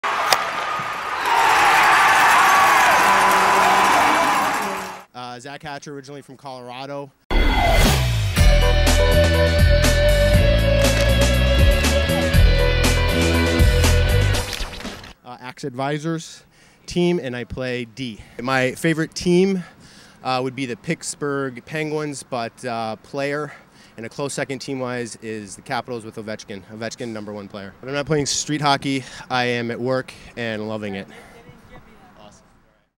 Penguins fan for Ovechkin
Zach Hatcher from Colorado picks Ovechkin as the top NHL player.